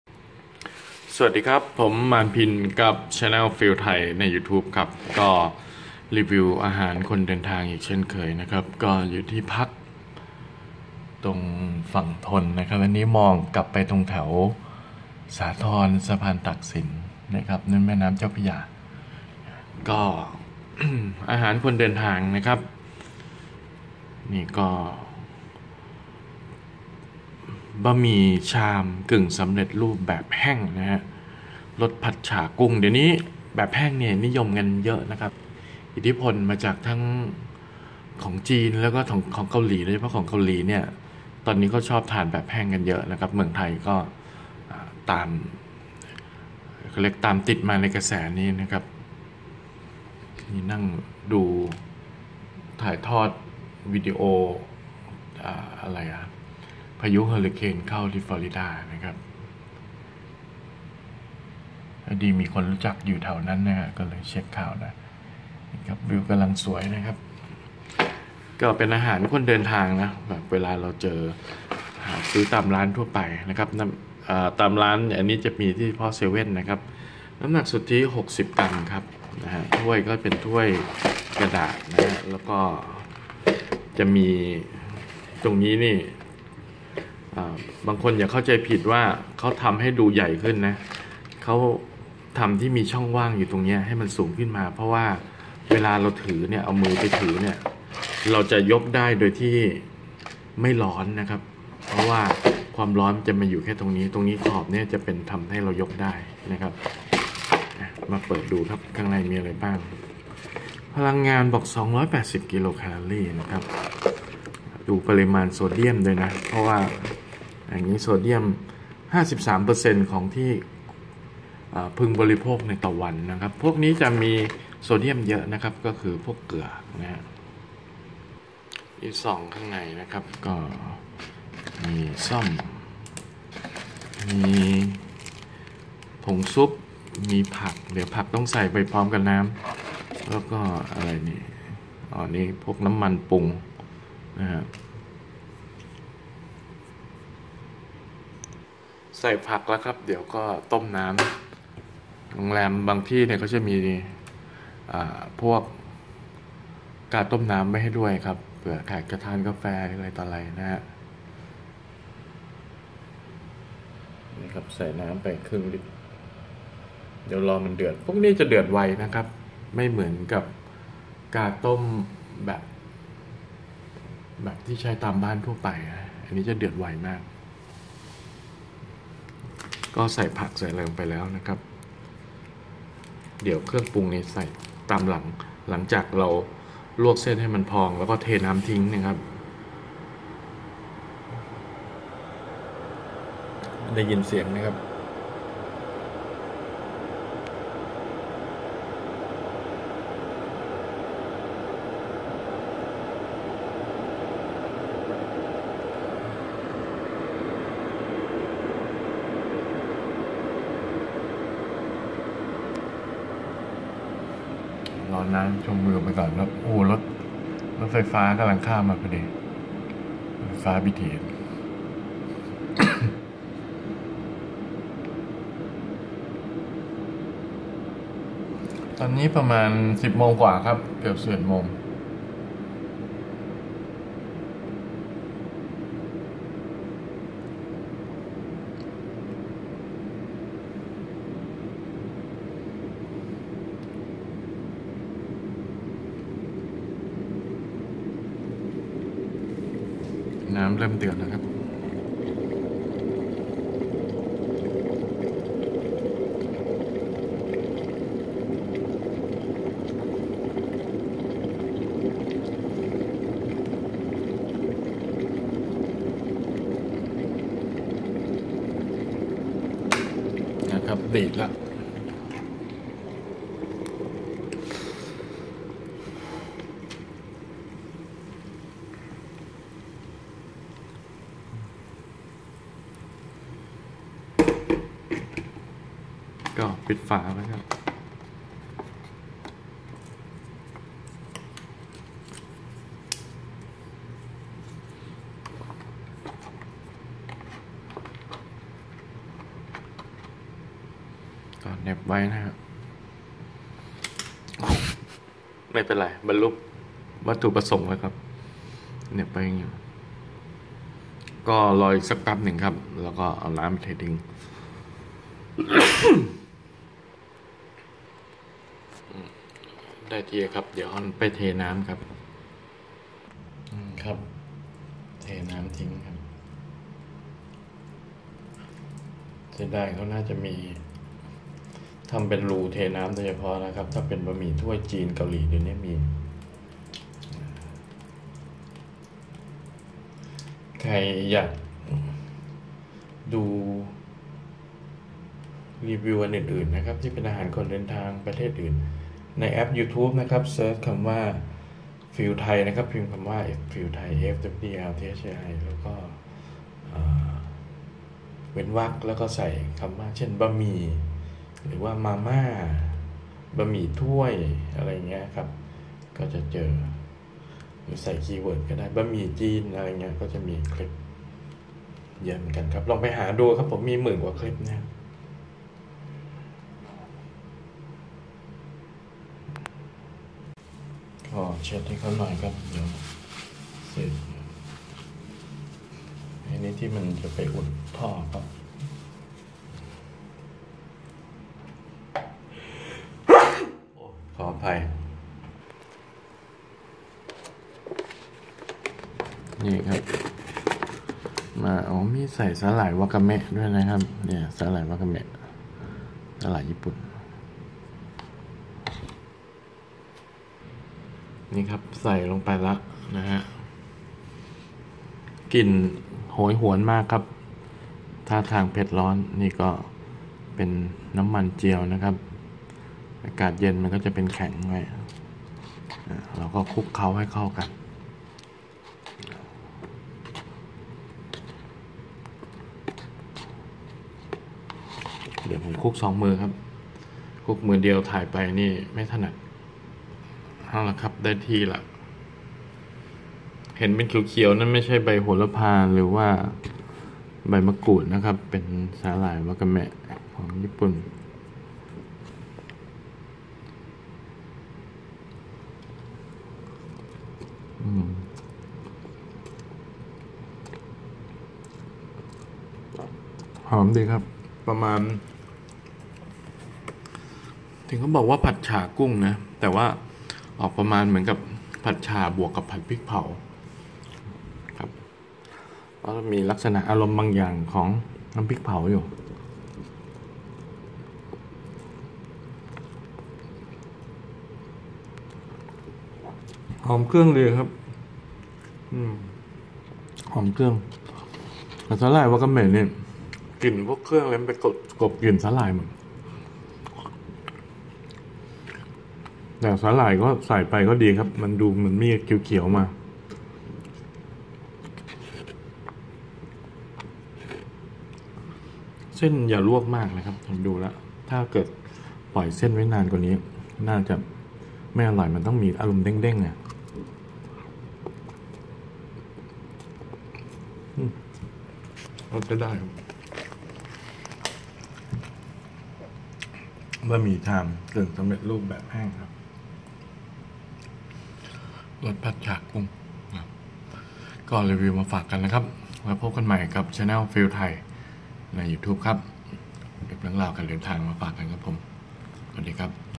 มาม่ารีวิว EP.51 บะหมี่เซเว่น รสผัดฉ่ากุ้ง |บะหมี่ถ้วย
บะหมี่เซเว่น รสผัดฉ่ากุ้ง |บะหมี่ถ้วย  รสชาติแบบแห้ง  เทน้ำทิ้งก่อนคลุกเครื่อง อร่อยดีครับ